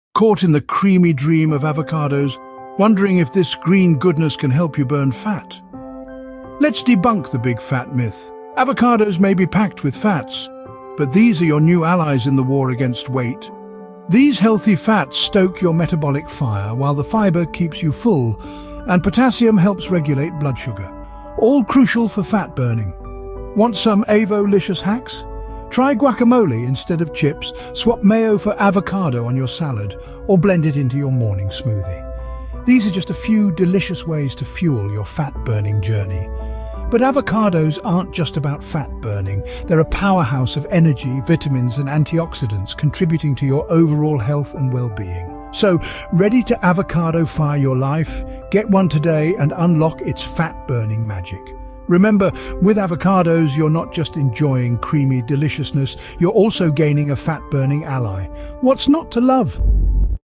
Avocado: The Creamy Dream Fuel
Forget the Fad Diets, Avocados Got Your Back!
Ditch the myths and unlock the fat-burning secrets of everyone's favorite creamy green fruit! This YouTube Short proves avocados are more than just delicious - they're your new BFF on the road to a healthier, happier you.

Unmask the science behind avo's magic:  Healthy fats that boost your metabolism like a dream
Fiber that keeps you fuller longer and cravings at bay
Potassium that regulates blood sugar, making fat burning smoother than ever

Skip the greasy chips and mayo - we're talkin' guac galore, healthy avocado toast, and even guilt-free smoothie power! Plus, avo's got your back (and energy levels) beyond just burning fat.